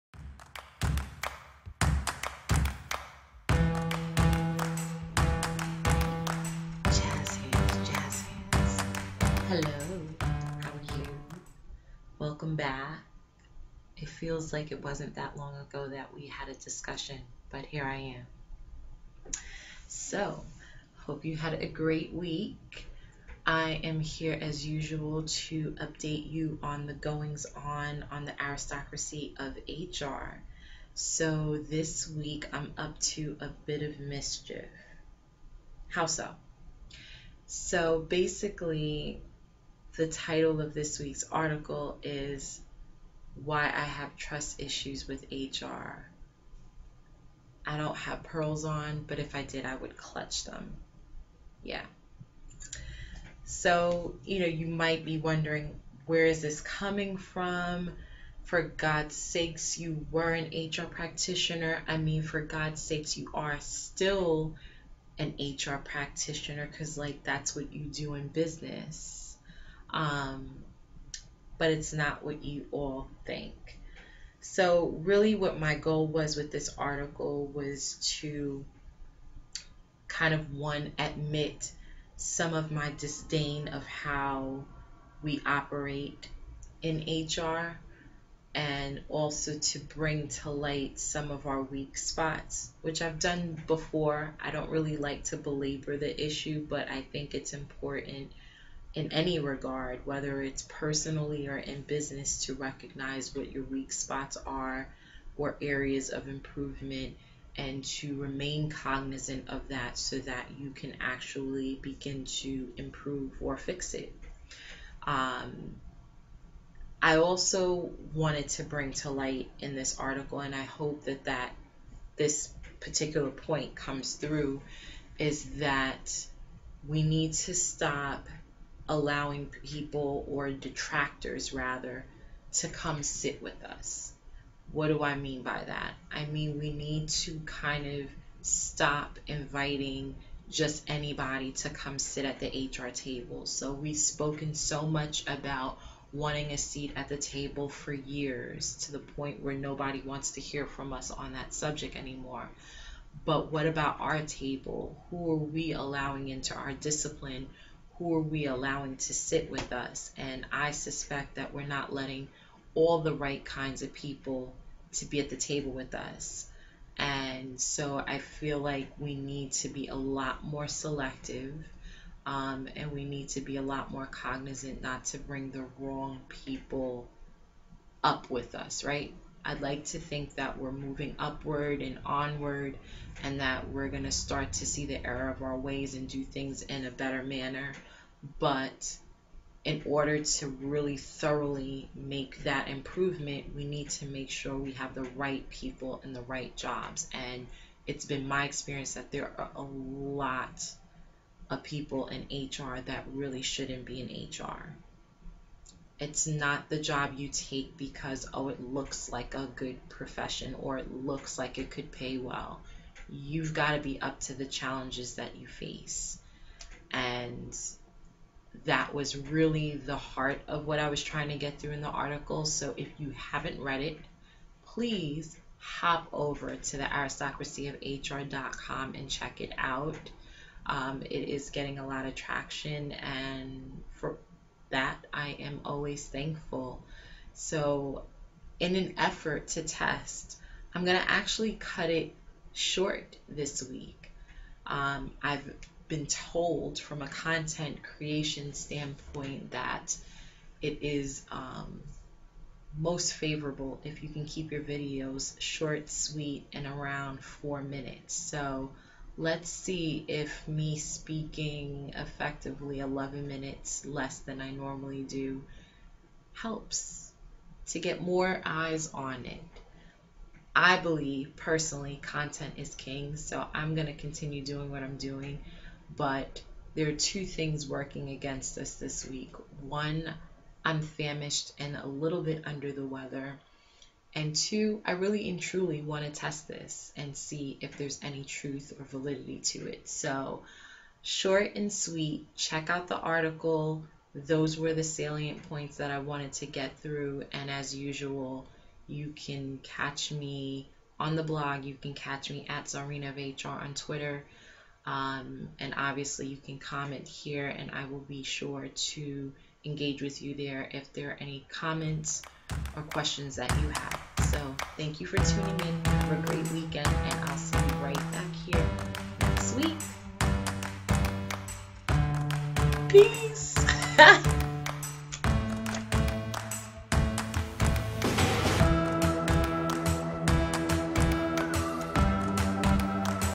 You can't sit with us: Trust Issues and HR
This video is a supplement to my recent article "Why I Have Trust Issues with HR. I talk about the main ideas I hope readers received in the article. To get caught up with The Aristocracy of HR's weekly articles, head over to thearistocracyofhr.com.